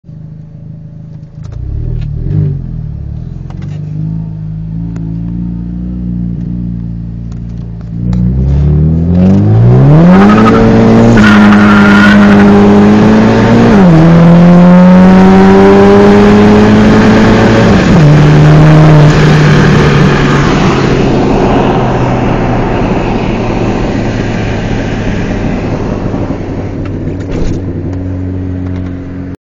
Solstice GXP 1-2-3 NLS Shift@WOT
Solstice GXP 1-2-3 NLS pull.  Tuned via HPT, Meth Injection, nitrous, 3" dump, etc.  2nd gear is just about useless.